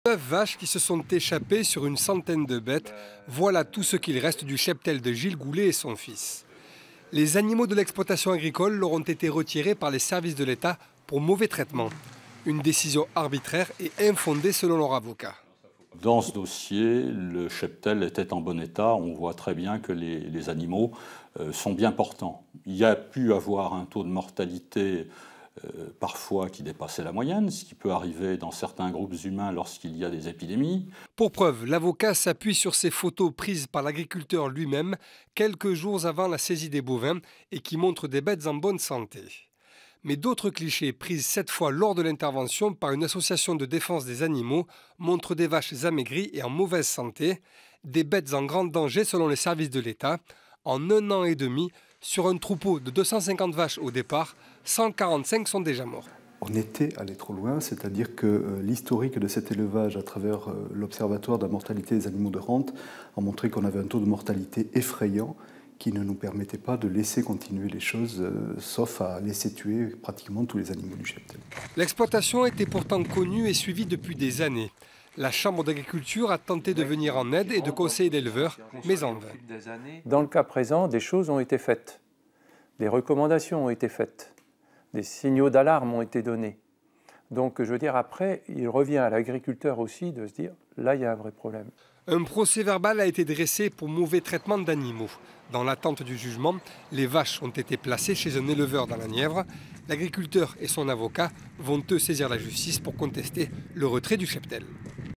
Ligny-le-Châtel : pourquoi un troupeau de bovins a été saisi par l'Etat ?
Un troupeau de bovins a été saisi par l'Etat la semaine dernière à Ligny-le-Châtel dans l'Yonne. Une procédure rare pour des animaux en réel danger selon les services de l'Etat. L'éleveur pourrait déposer le bilan de son exploitation agricole mais conteste cette décision.